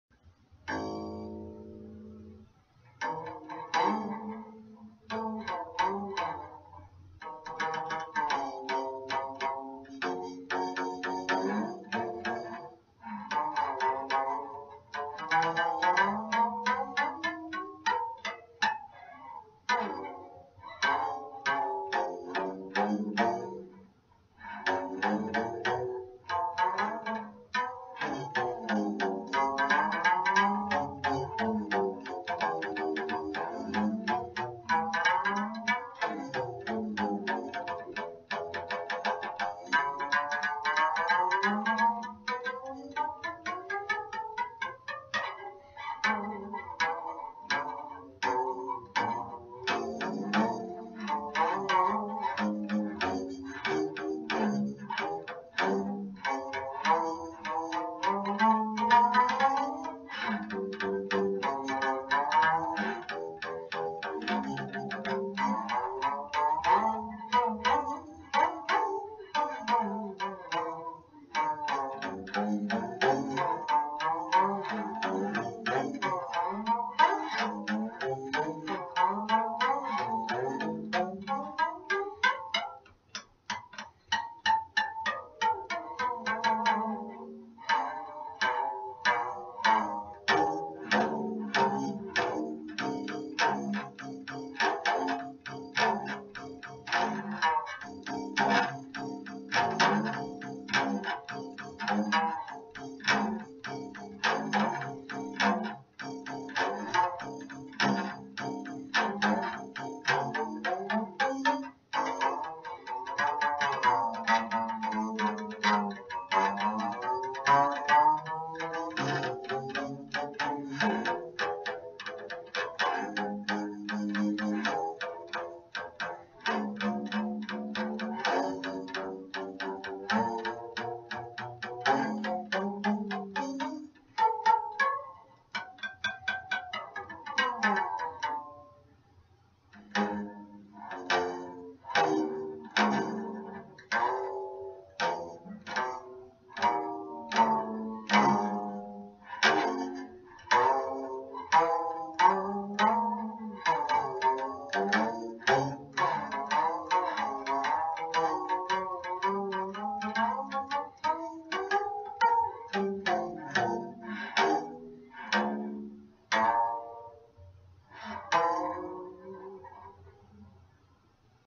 JOHN TOILET BOWL vs 2 STRINGS BASS-BOX
I will continue to practice even on my JOHN TOILET BOWL with my 2 STRINGS BASS-BOX ...